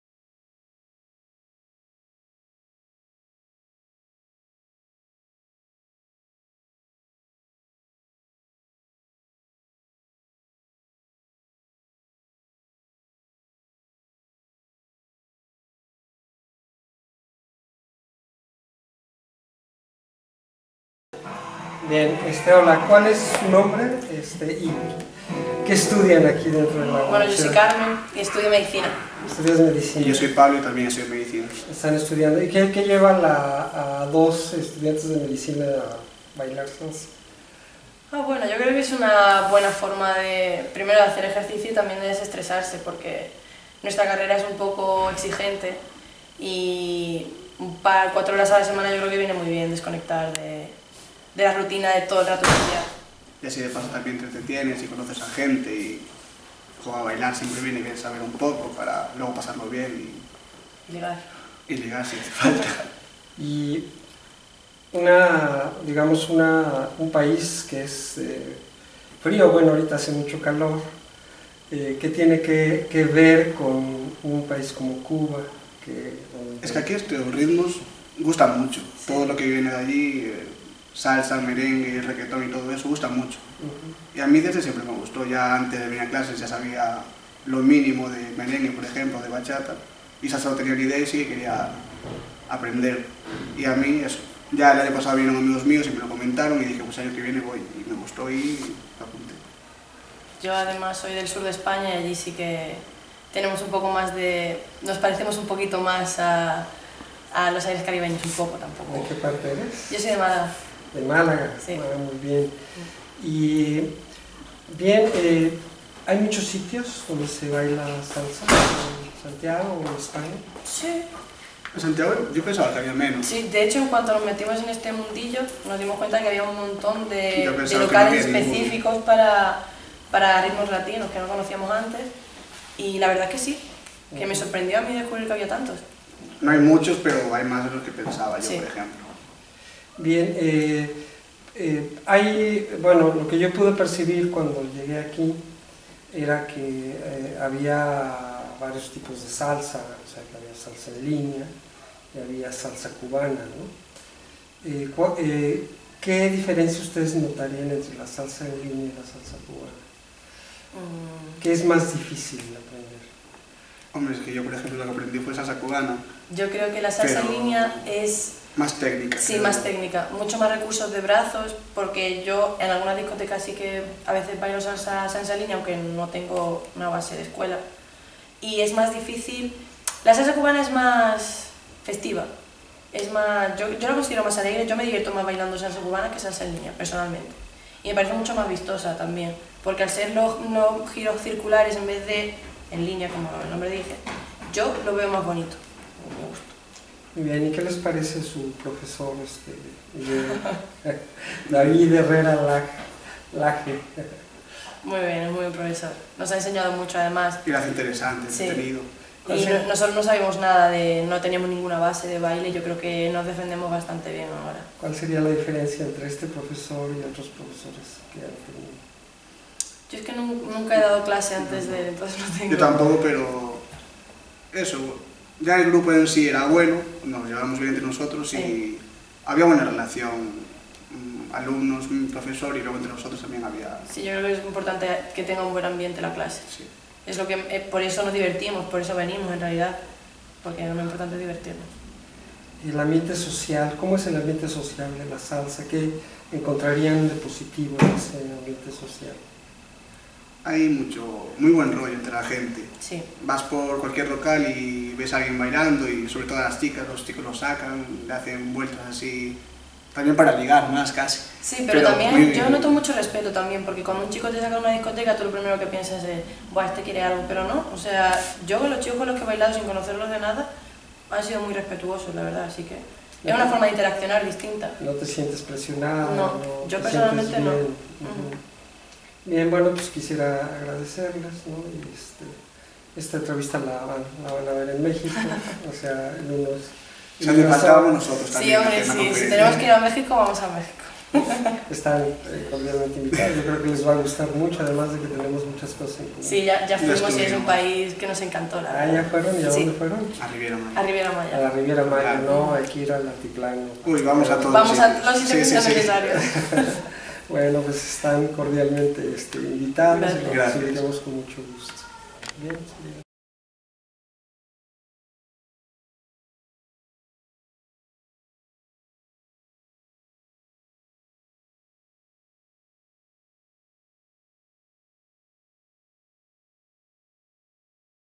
Entrevista alumnos USC sobre la Salsa Cubana
Carmen Bermudez Hormiga y Pablo Salgado Rodríguez respondieron preguntas del Mtro. Arturo Luis Alonzo Padilla sobre el aprendizaje de la salsa cubana que cursaron en la Universidad de Santiago de Compostela, como parte de las actividades culturales que esta universidad desarrolla. En la era global, los ritmos latinos penetran en Europa y gustan a los jóvenes, la Universidad les da la oportunidad de aprenderlos. Bailar ritmos latinos es una actividad muy sana como lo dicen los estudiantes y en un ambiente de respeto, sin drogas y sin adicciones.